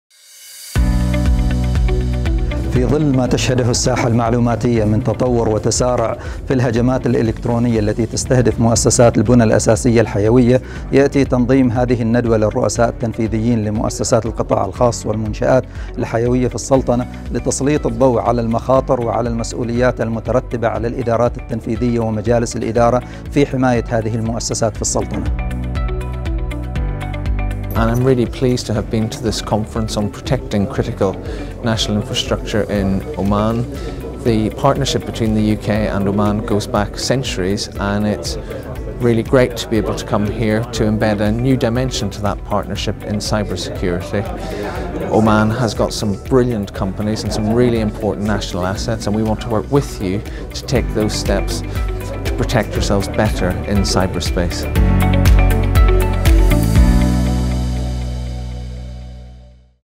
لقاءات من | ندوة الرؤساء التنفيذيين لحماية المنشآت الحيوية من المخاطر الإلكترونية
لقاءات من ندوة الأمس التي ناقشت موضوع حماية الـمنشآت الحيوية من مخاطر الأمن السيبراني.

Interviews from the Critical National infrastructure CyberSecurity seminar that was held yesterday.